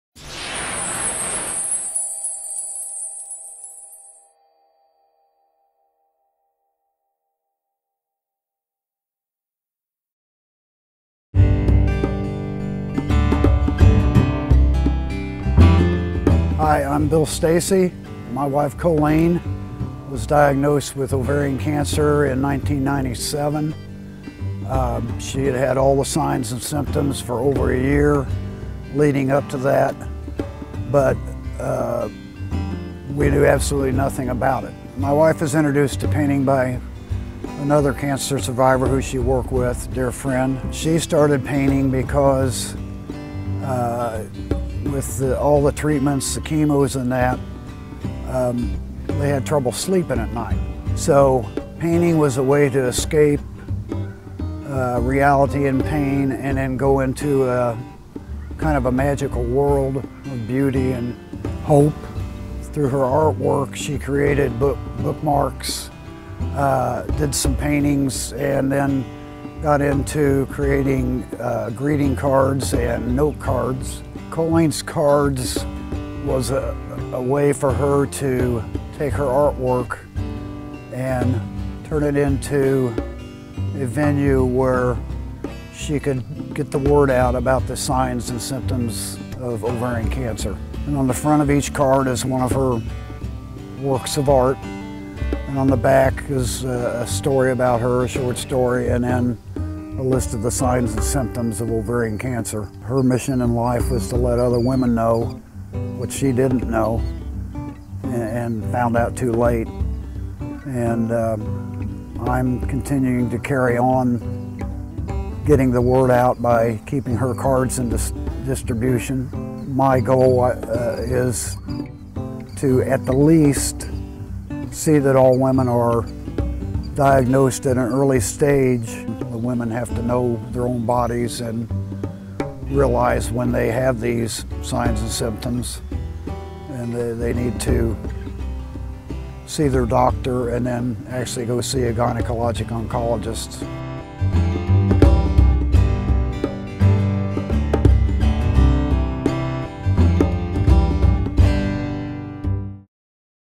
Kolleen's Kards - About
Bill Stacey, Co-Founder of "Kolleen's Kards" gives a brief description and history of what "Kolleen's Kards" is all about.
To learn more, and to order some of Kolleen's Kards, be sure to visit KolleensKards.org